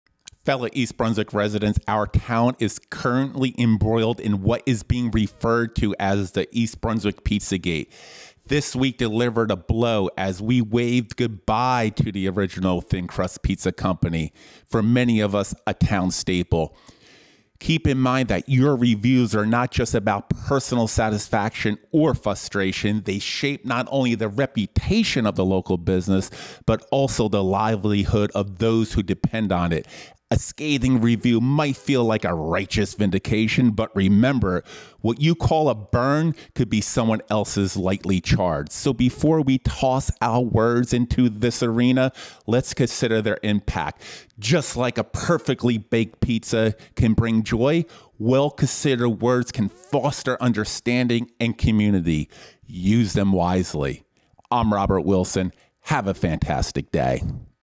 Robert Wilson Discusses the East Brunswick PizzaGate Controversy.
In this short, Robert Wilson dissects the spicy topic of the East Brunswick PizzaGate kerfuffle. The shutting down of "The Original Thing Crust Pizza Co." caused tremors that reached the Richter scale in East Brunswick, stretching all the way to the far reaches of Middlesex County. Operating a business is as smooth as a gravel road, folks. You might have the most heavenly pie and dazzling service, but sometimes, that's as effective as a chocolate teapot when it comes to ensuring victory.